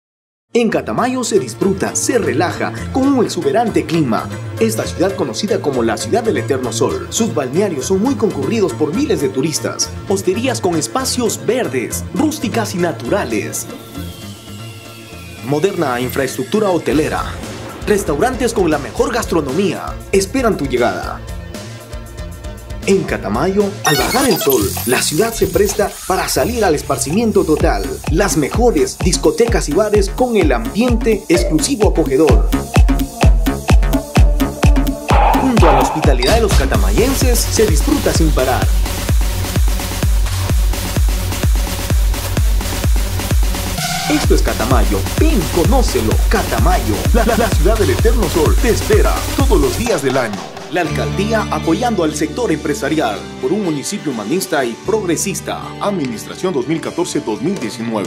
Alcaldía de Catamayo.- Infraestructura Turística de Catamayo
Catamayo la ciuduad del eterno sol, en la regio sur del Ecuador, tiene todo lo que usted necesita para vivir momentos inolvidables, en un ambiente natural, con su clima seductor..!

En Mayo Tu destino es Catamayo